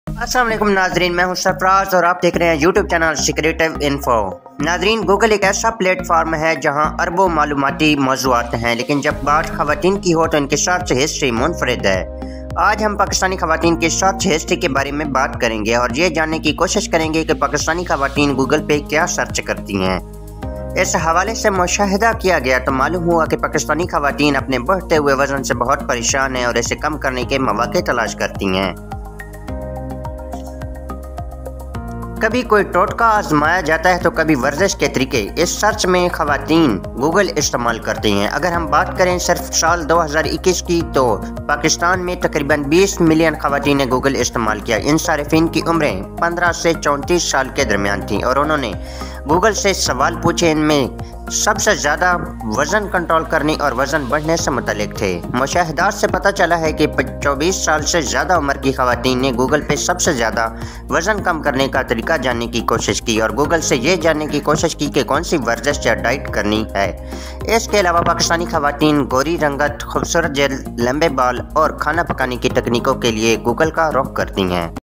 What Pakistani's Girls Search Most On Google.?||Pakistani women
In this video I'm going to explain that What Pakistani women search most on Google.